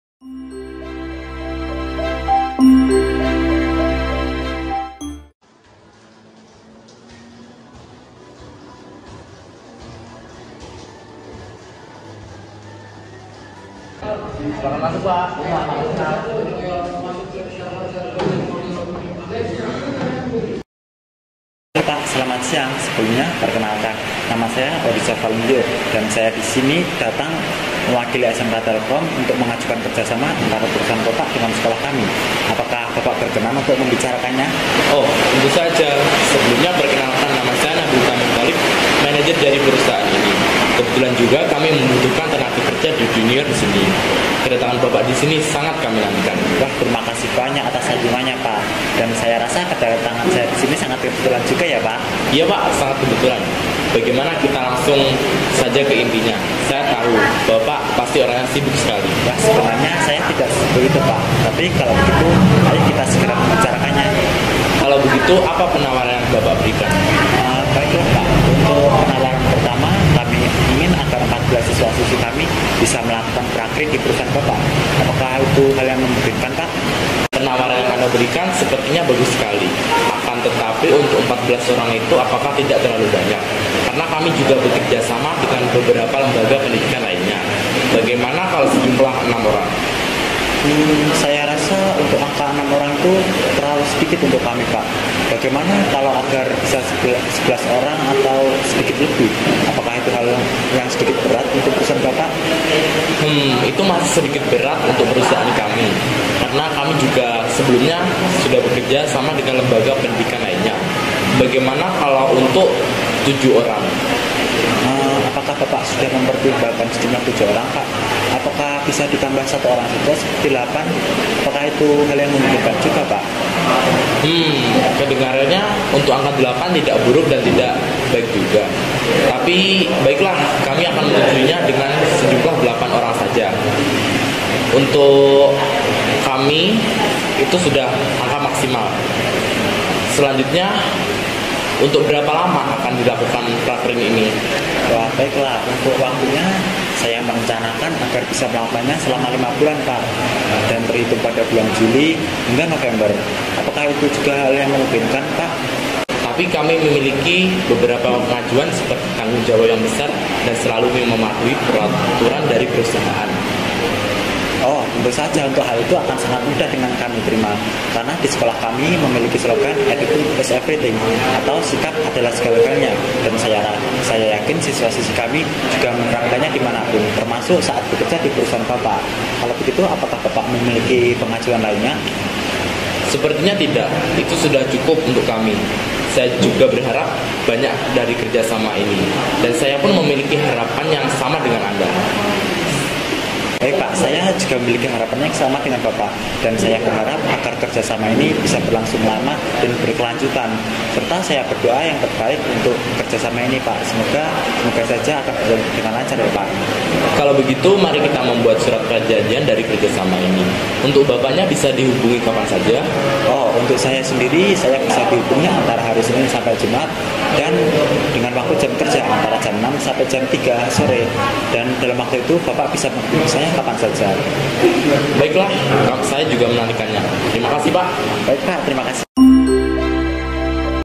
Kerja sama sekolah dengan perusahaan
Nabil Tamim Thalib (24)
Oryza Valendio (29)

video ini adalah video negosiasi kerja sama antara pihak sekolah yang diperankan oleh Oryza Valendio dan perusahaan yang diperankan oleh Nabil Tamim Tholib. Pada video ini pihak sekolah menegosiasikan sejumlah siswa yang akan melakukan prakerin di perusahaan tersebut dan berapa jangka waktu yang akan dilakukan.